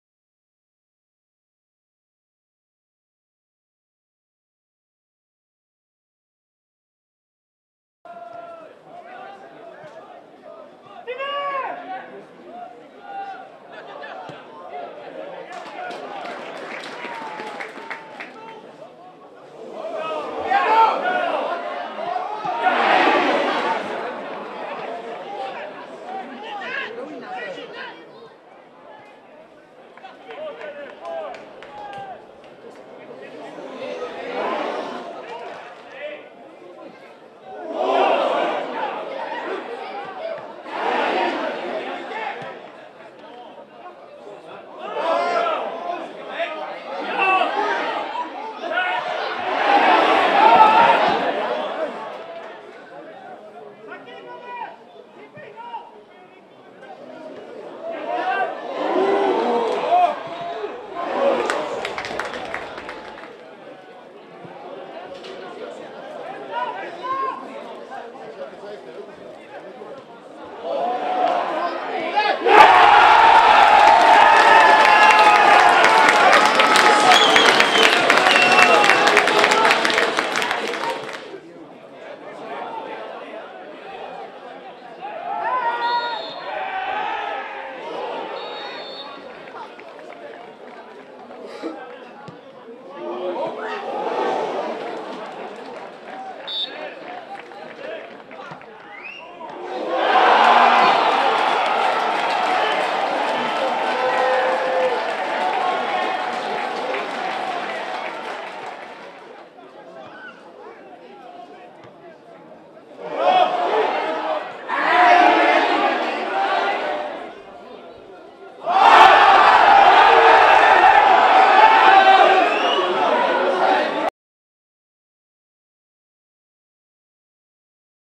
Highlights fff-jönköping.mpg
Highlights från mötet mellan Falkenbergs FF och Jönköpings Södra IF i Superettan 2010.